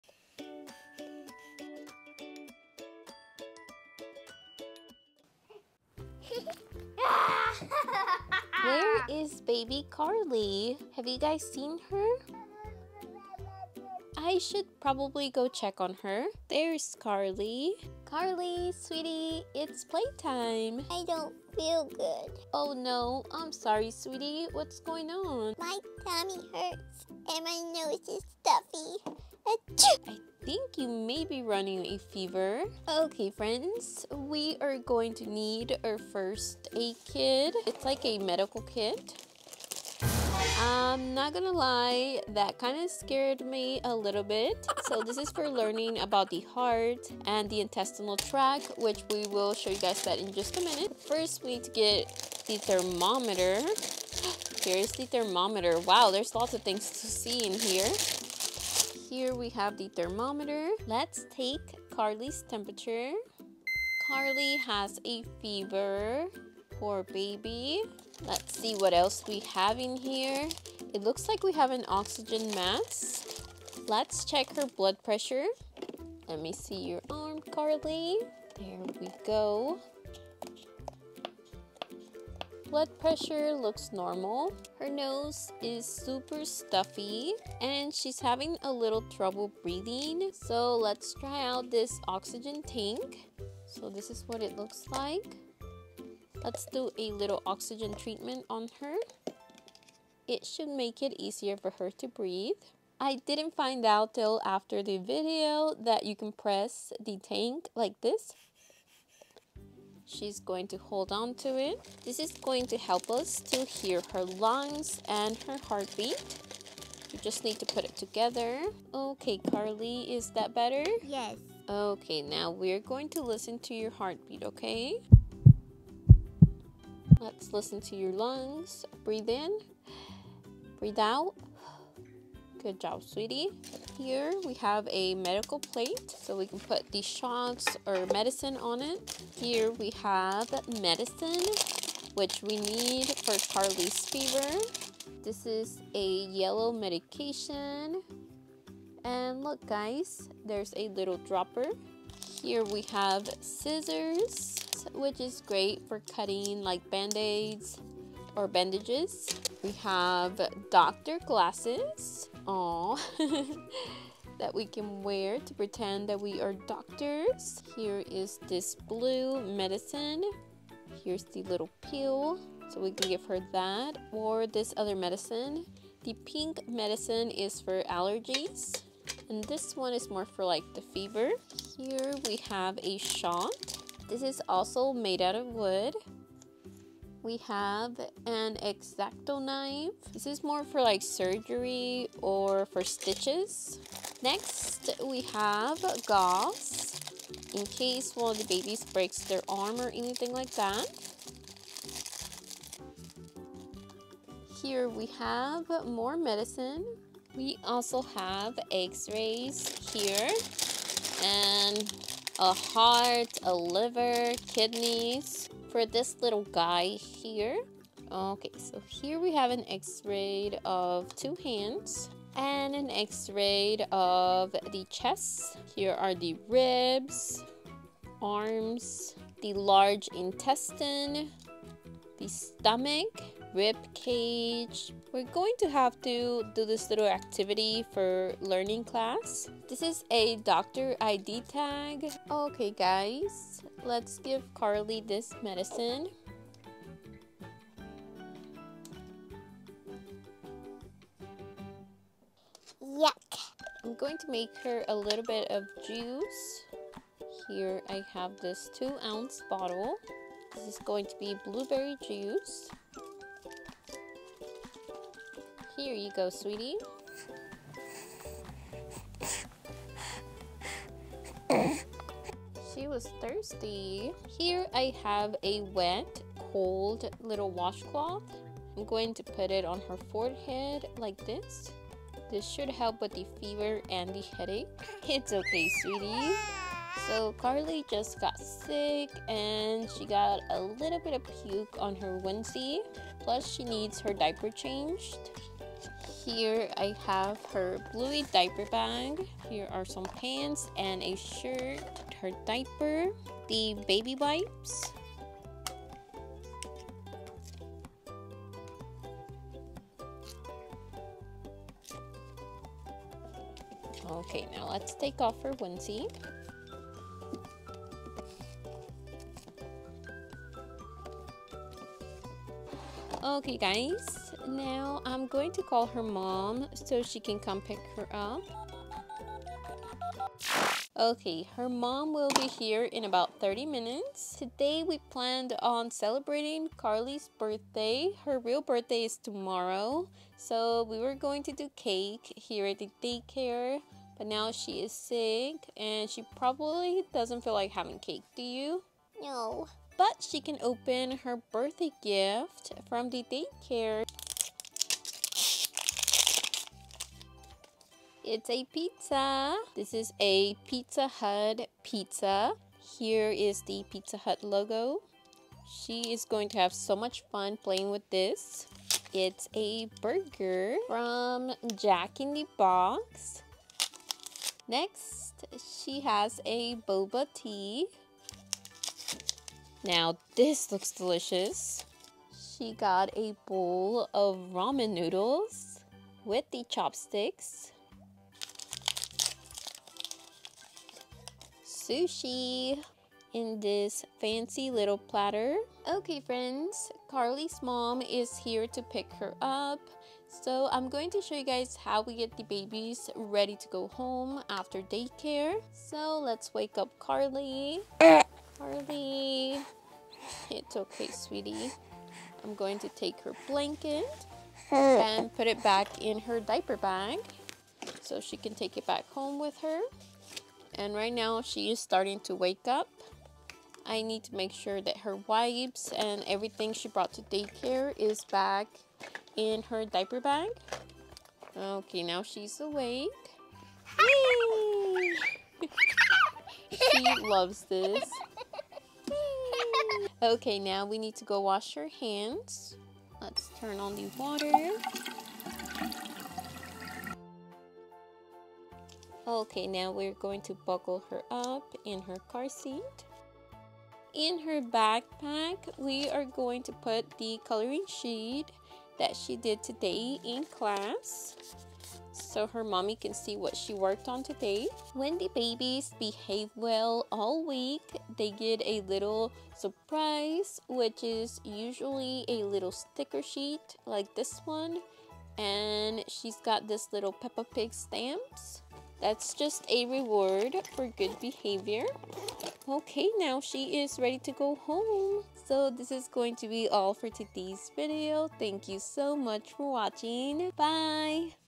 Baby Born doll Cold day Routine and check up with toy doctor kit
Baby Born doll Cold day Routine and check up with toy doctor kit at baby doll daycare care center for baby dolls pretend play doll videos for toddlers